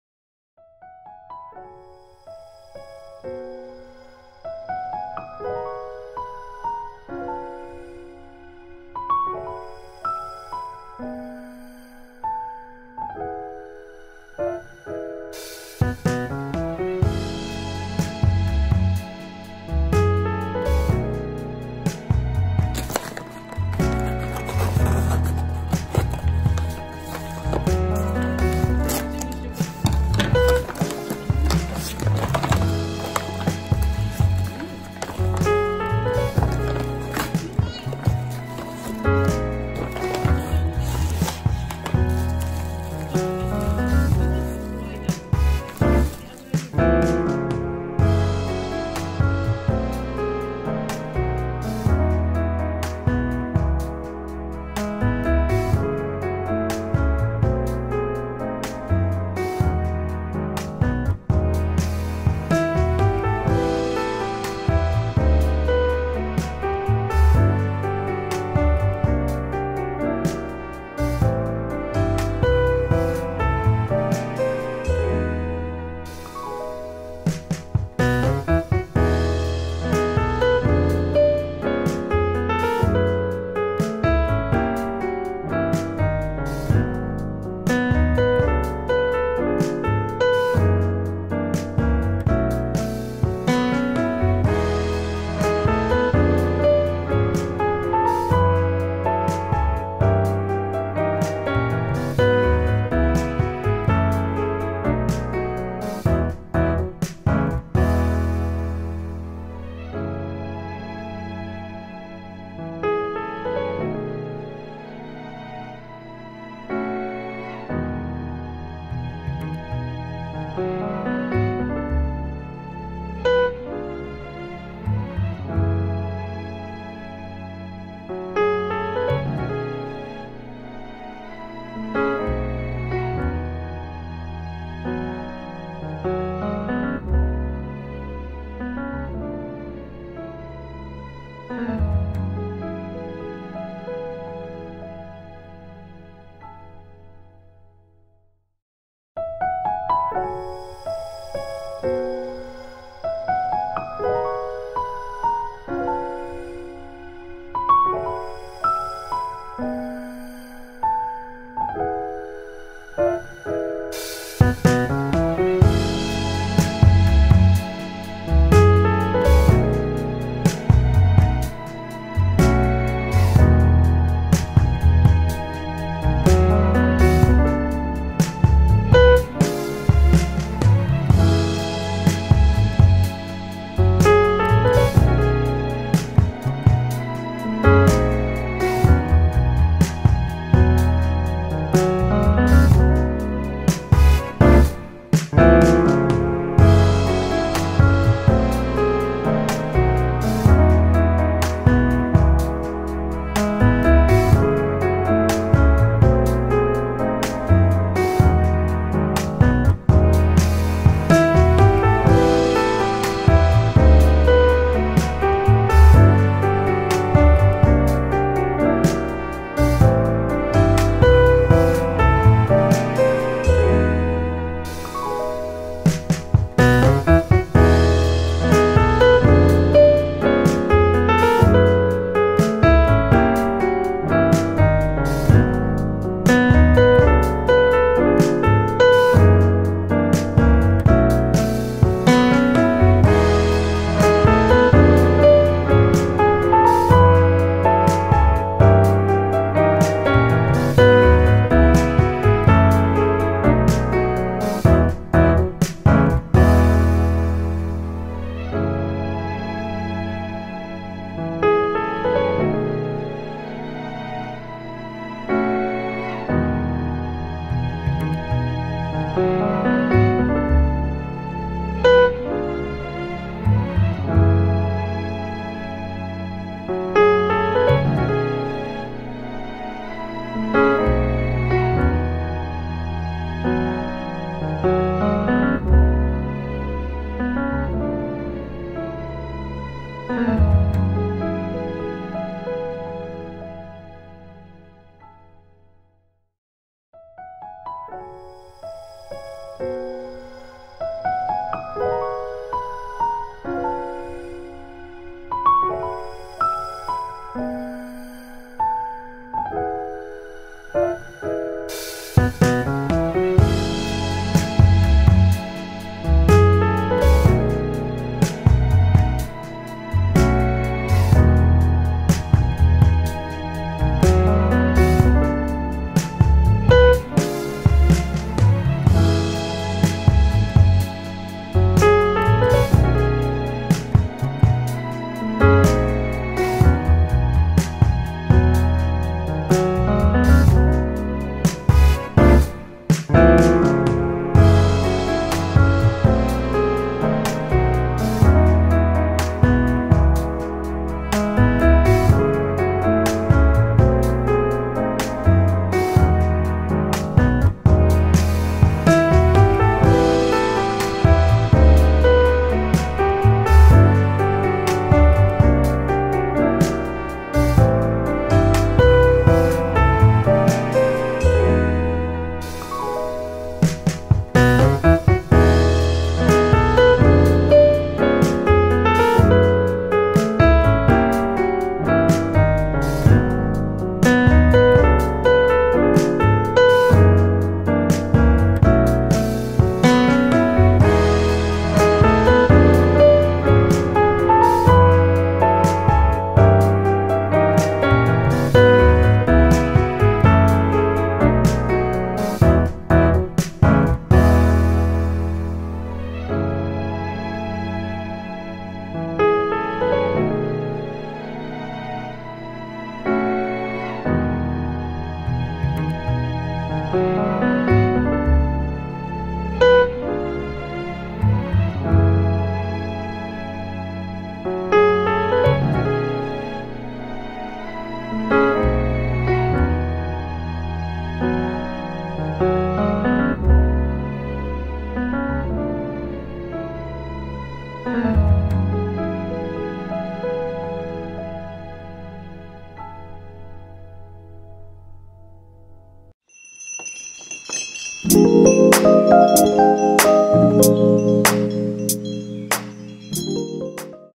【購入品】プチプラ半額セール ネイル用品紹介
暇ネイルです。
本日は、この前の楽天スーパーセール時に
プチプラさんで半額セールを行っていたので、たくさん購入しました🥰💸💸💸
そちらの購入品動画を撮ってみたので、みなさん是非見てください😌！